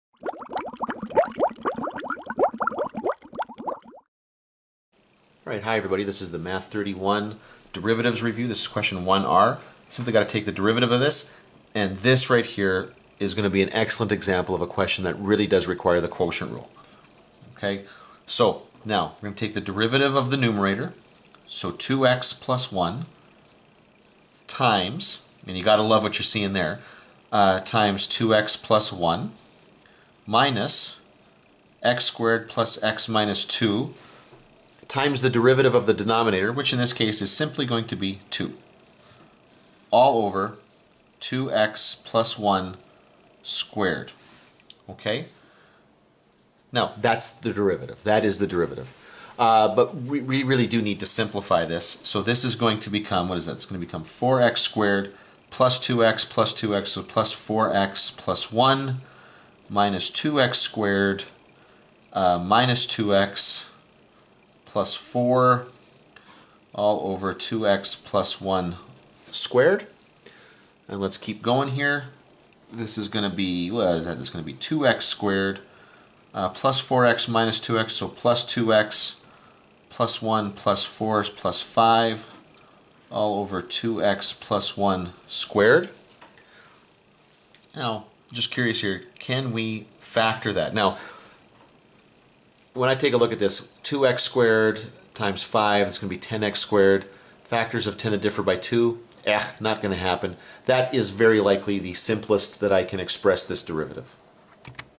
Math 31 Derivatives Review
This is about finding the derivative of a function.